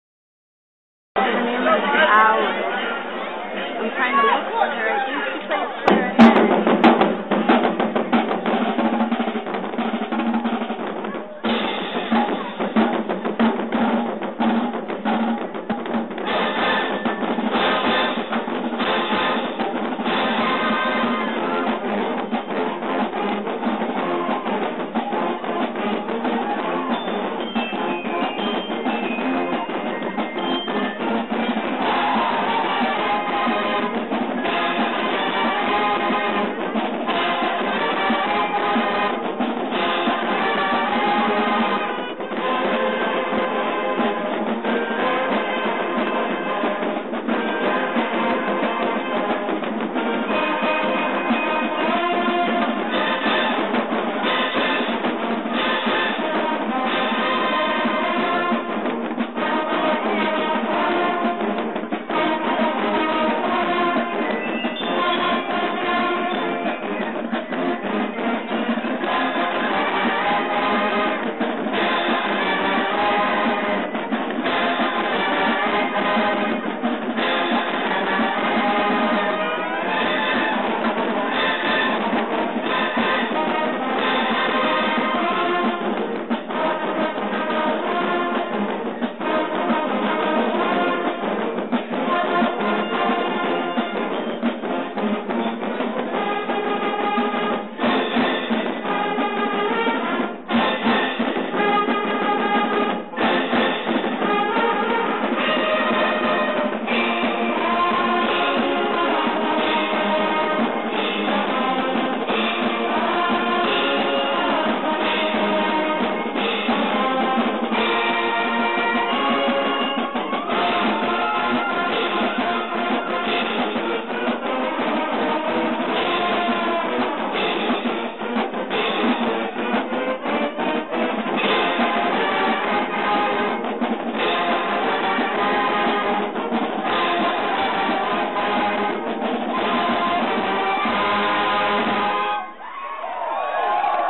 UC Davis Las Vegas flash mob performance song 2
I was in Vegas with my girlfriend, when a group of 100 UC Davis band members performed a flash mob of sorts right on the streets of the strip!